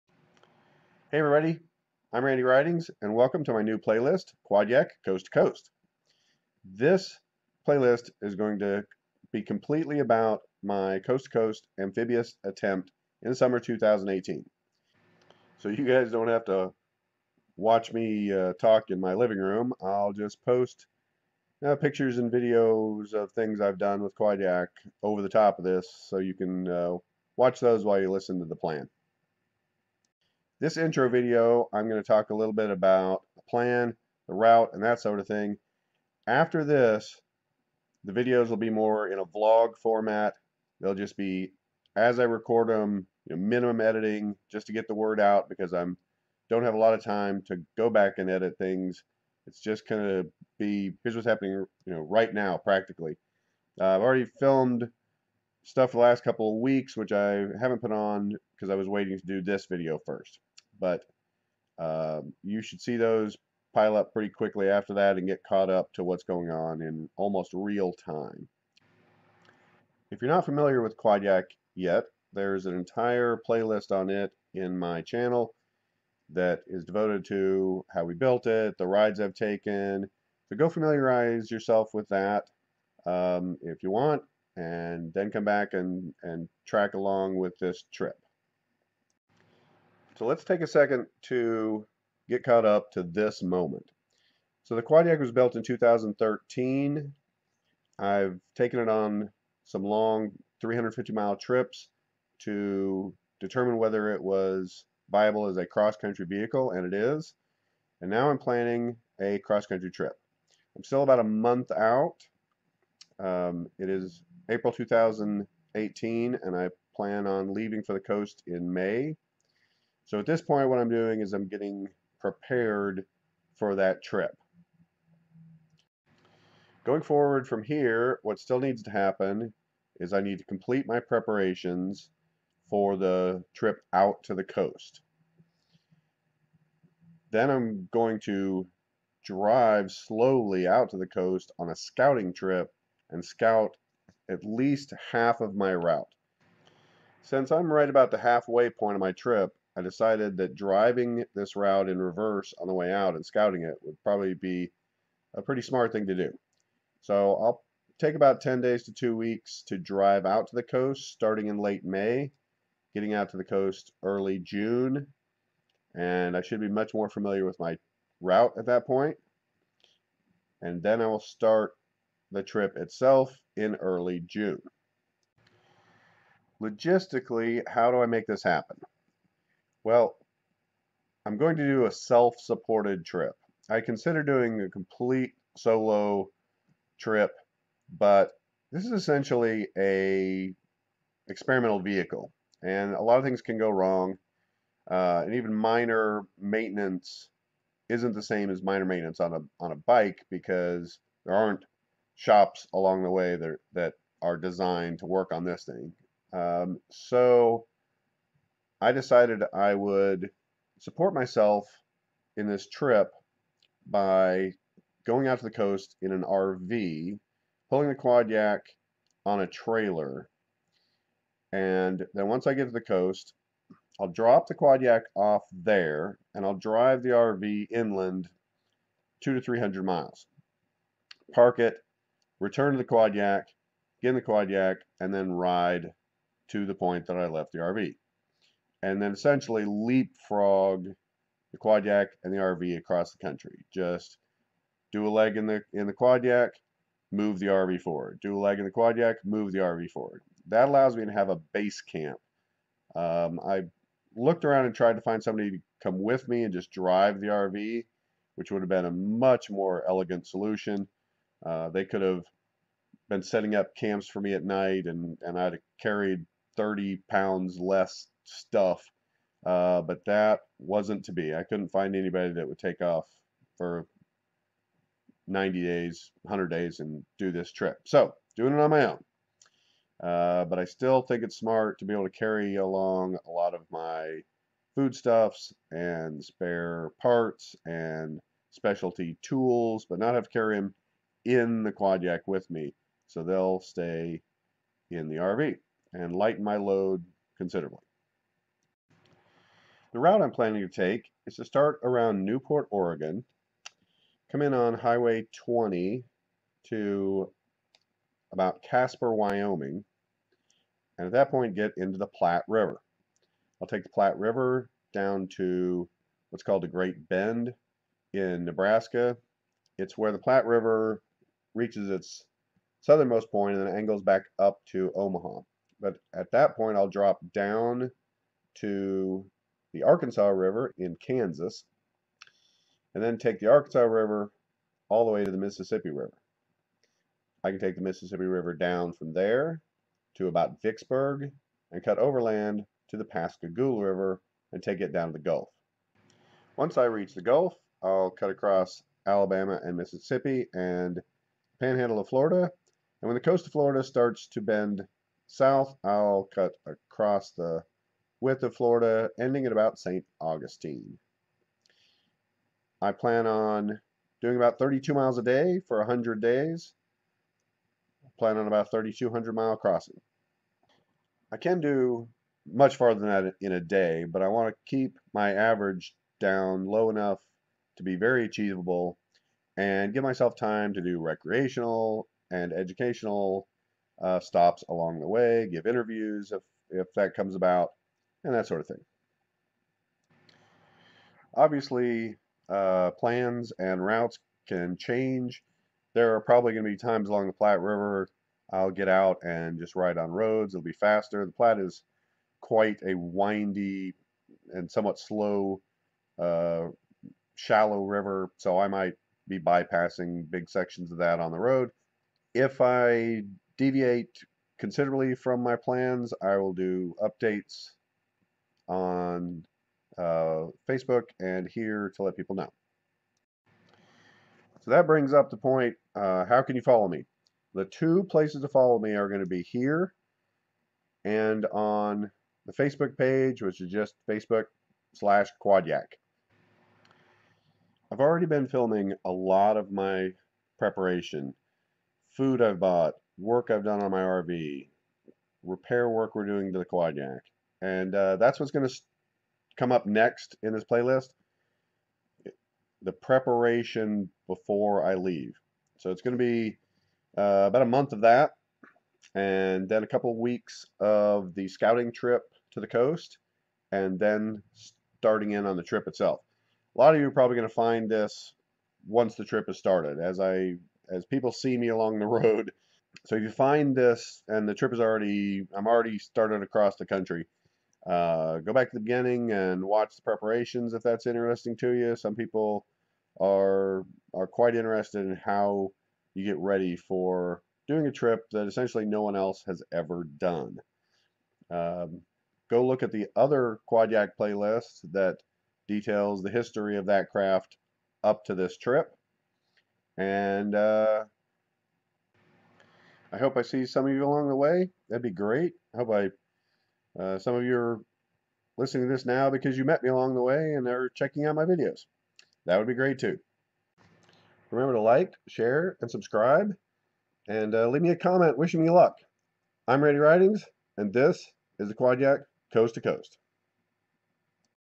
Quadyak Coast to Coast Ep1 Intro Human Powered Amphibious Trip Across America Summer 2018
Introducing the playlist that will cover my attempt at crossing the U.S. in my human powered amphibious recumbent, the Quadyak, in the summer of 2018.  I detail the plan, timeline, route and how to follow along! I will be the first person to accomplish a human powered amphibious crossing of America if I succeed.